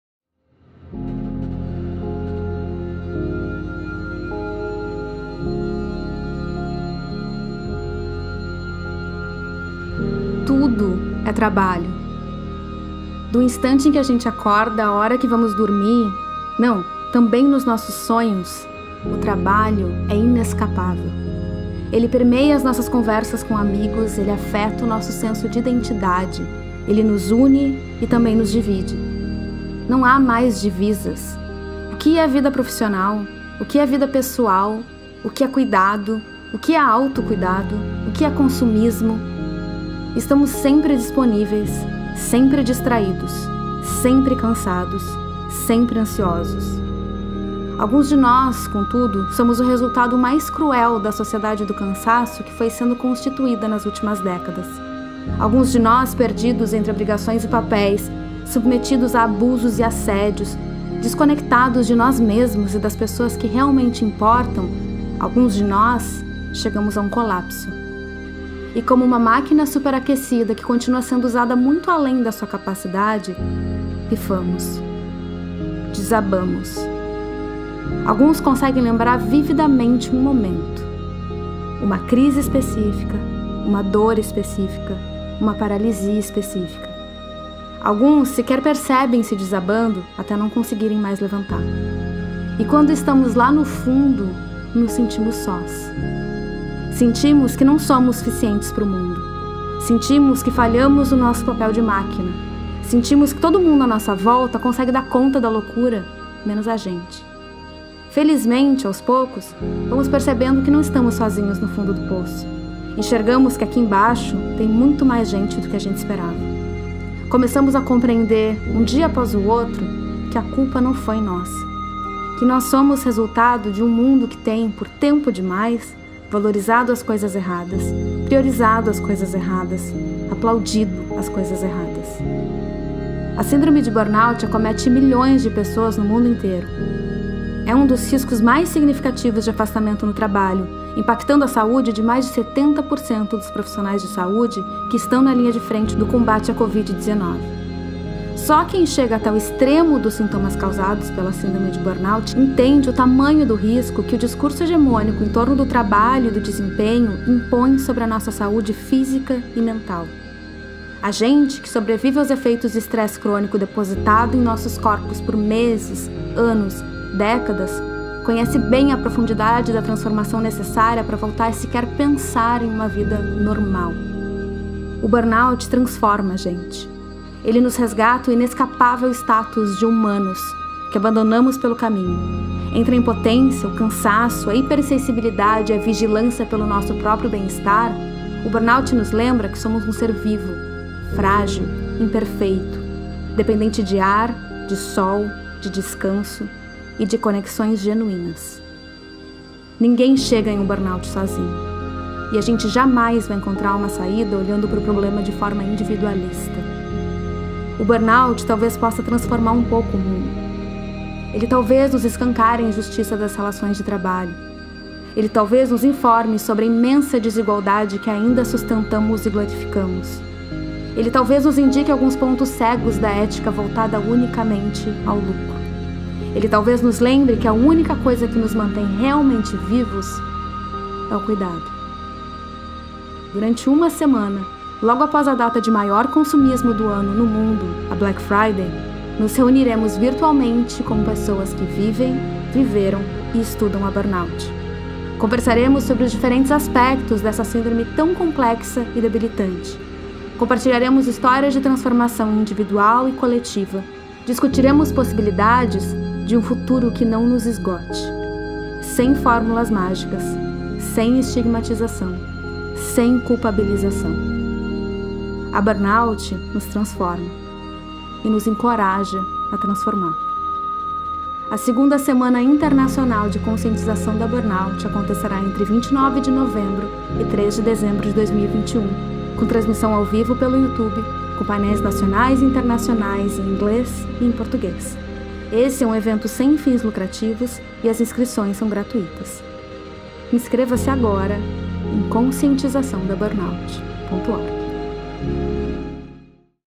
Por um futuro que não nos esgote. • II Semana Mundial da conscientização da Burnout (2021)
Ninguém chega em um burnout sozinho.
E a gente jamais vai encontrar uma saída olhando pro problema de forma individualista.

A II Semana Mundial de Conscientização da Burnout acontecerá entre 29 de novembro e 03 de dezembro, com transmissão ao vivo pelo Youtube, com painéis nacionais e internacionais, em inglês e português.
Este é um evento sem fins lucrativos, e as inscrições são gratuitas.

Produção e Edição: Etiene Rocha
Narração e Roteiro: Carol Milters